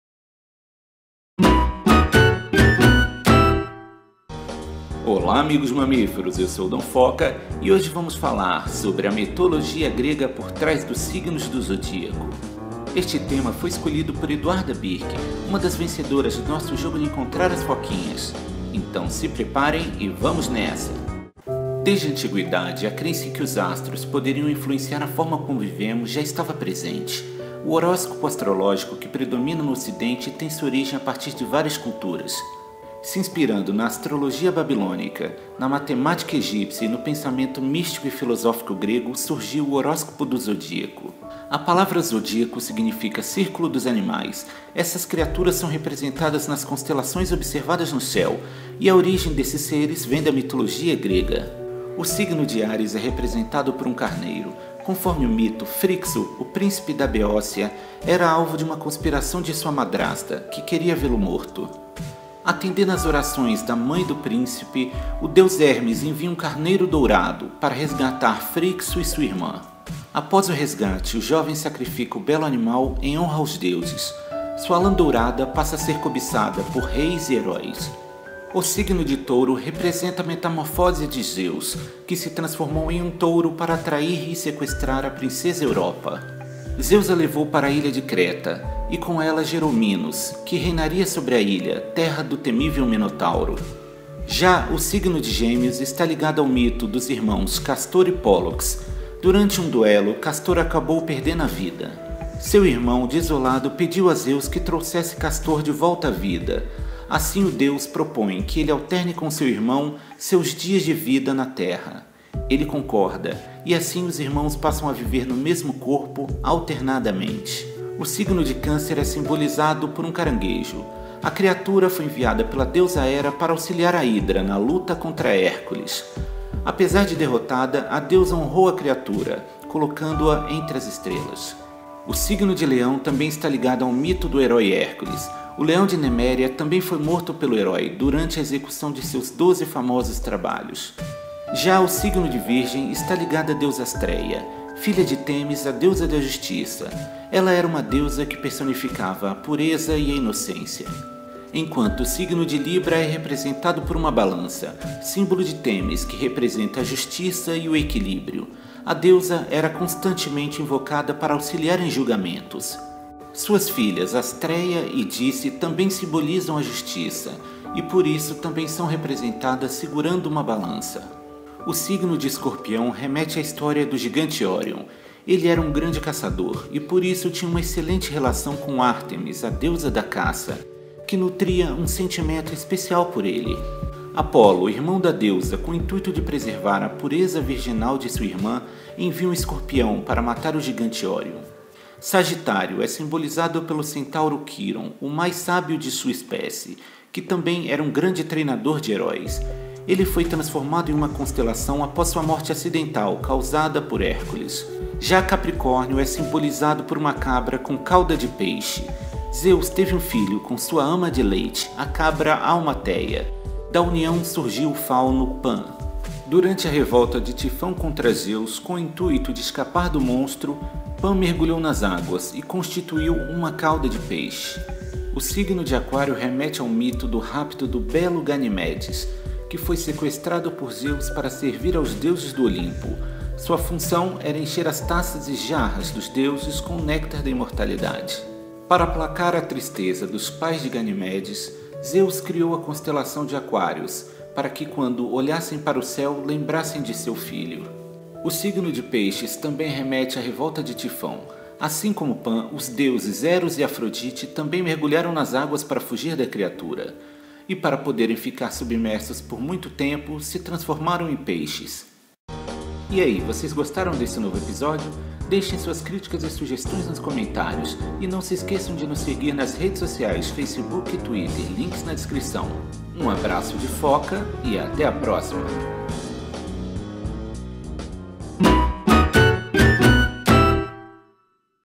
Os signos do Zodíaco e a Mitologia Grega - Curiosidades Mitológicas #06 (Foca na História)
Conheça a relação entre os signos do zodíaco e a mitologia grega. A astrologia que envolve seu horóscopo está muito mais ligada aos Deuses do Olimpo do que você imagina.